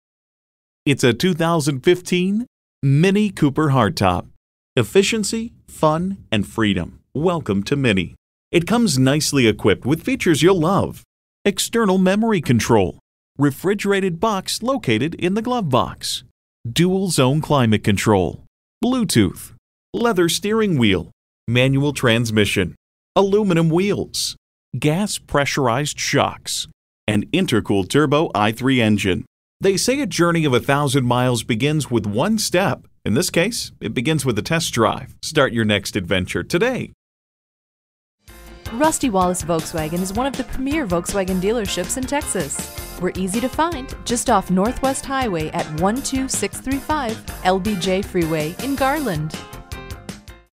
Used 2015 MINI Cooper Dallas TX Garland, TX V210475A - SOLD
Year: 2015
Make: MINI
Model: Cooper
Trim: Base
Engine: 1.5L 12V TwinPower Turbo
Transmission: 6-Speed Manual
Color: White
Interior: Carbon Black
Mileage: 58346
Stock #: V210475A
VIN: WMWXM5C51F3A96915

Address:
12635 Lyndon B Johnson Freeway